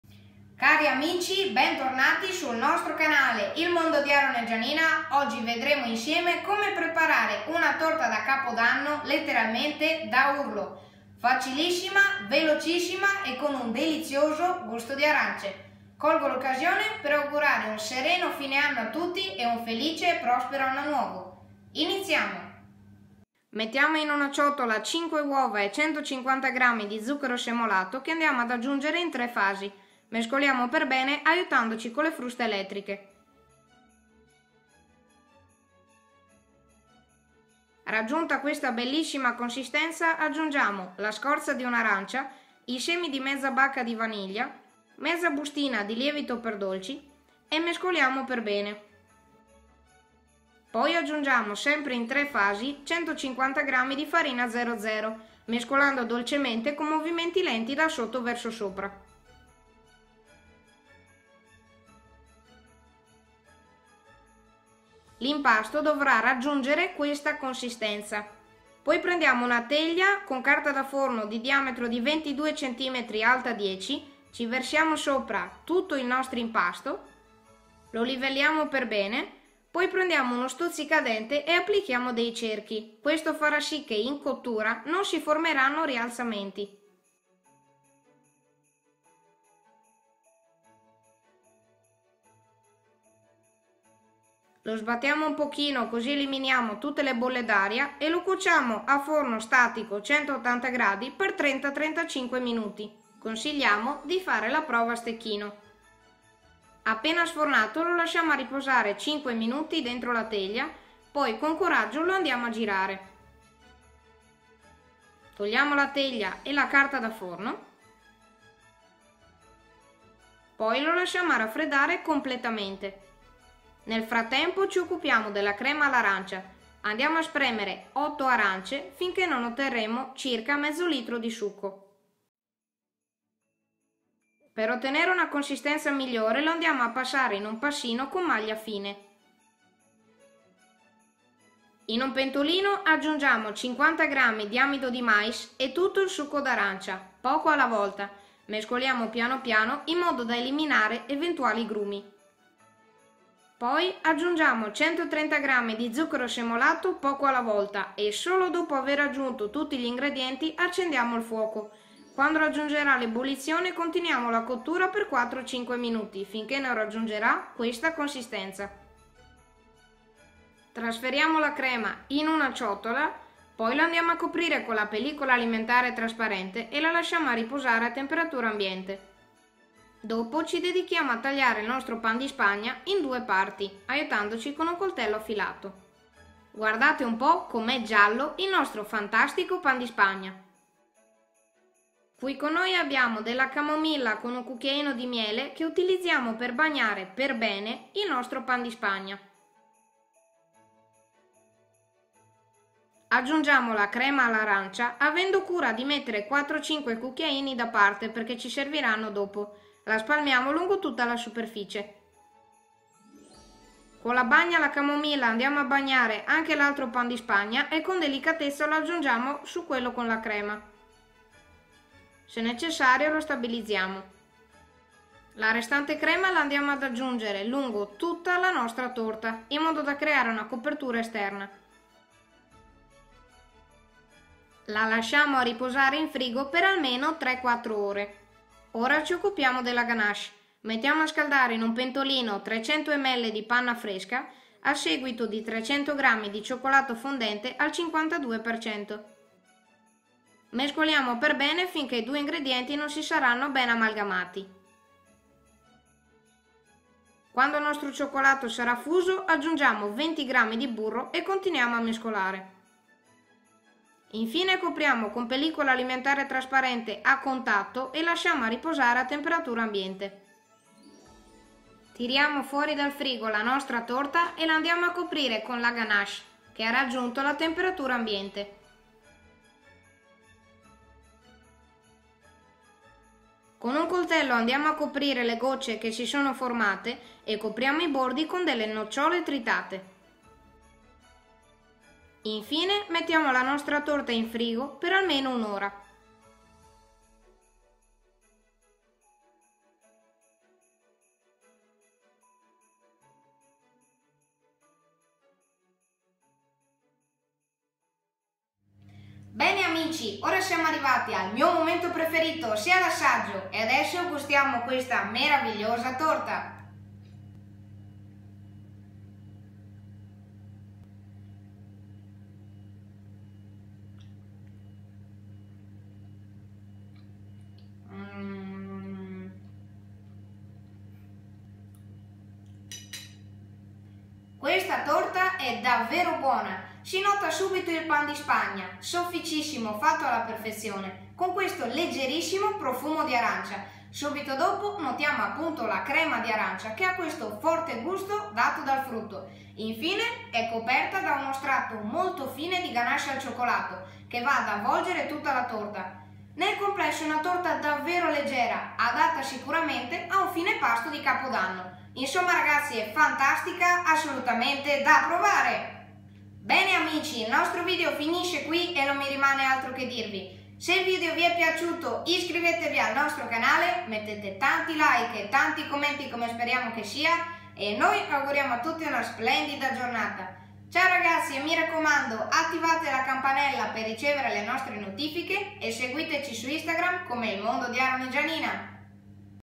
TORTA di CAPODANNO ALL'ARANCIA
Per chi a piacere di sostenere il nostro canale, abbiamo deciso di usare uno strumento semplice e sicuro per tutti. Grazie di cuore a tutti voi

Ciao a tutti, oggi abbiamo il piacere di presentarvi una torta davvero fantastica per il cenone di capodanno: LA TORTA ARANCIA E CIOCCOLATO. È una torta facile, veloce e davvero bella; grazie alla sua leggerezza potrebbe tranquillamente essere servita come fine pasto di capodanno.
Questo sarà l'ultimo video dell'anno, quindi auguriamo a tutti un sereno fine anno e un felice anno nuovo!!

Per questa ricetta ci serviranno:
PAN DI SPAGNA
150g di zucchero
150g di farina "00"
5 uova
la scorza di un'arancia
i semi di mezza bacca di vaniglia
mezza bustina di lievito per dolci

CREMA ALL' ARANCIA
500ml di succo di 8 arance
50g di amido di mais
130g di zucchero semolato

GANACHE
300ml di panna fresca
300g di cioccolato fondente
20g di burro

BAGNA
camomilla
1 cucchiaino di miele